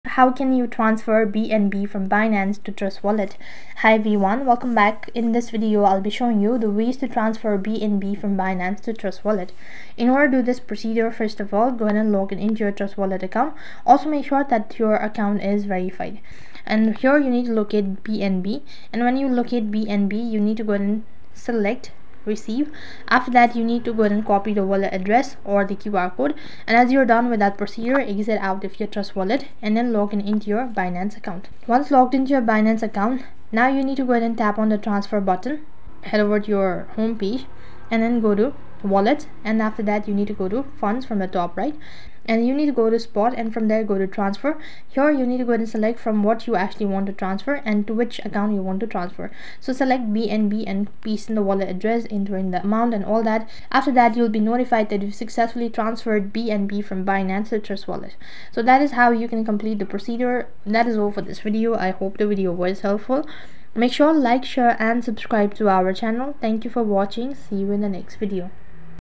How Can I Transfer Bnb From Binance To Trust Wallet !
This Video Is based on How you can Transfer Bnb From Binance To Trust Wallet.

Chapters / Timestamps
0:00 Introd
0:13 Transfer BNB From Binance To Trust Wallet
1:02 Outro